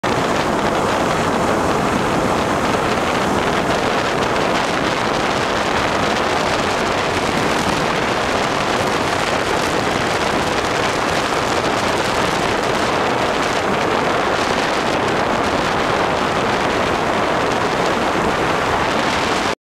Process heater Low Emission burner with no flame on right staged tips due to fouled primary tips
Fouled primary tips on this low emission burner case a lack of primary flame, thus the staged fuel flames have nothing to ignite them.
Where do you think the non-combusted fuel is going? (leave in the comments)